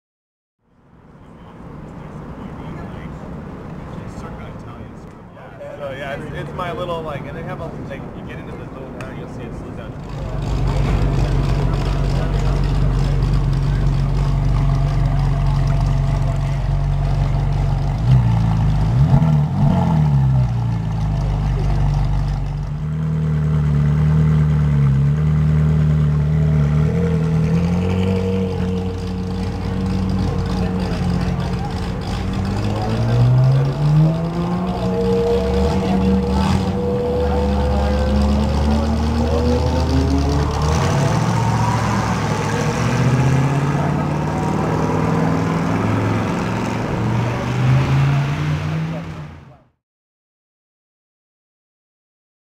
Ferrari 458 Italia
A red and black 2012 Ferrari 458 Italia leaving CNC Motors' Meet and Greet event.

(December 9, 2012 / Ontario, CA)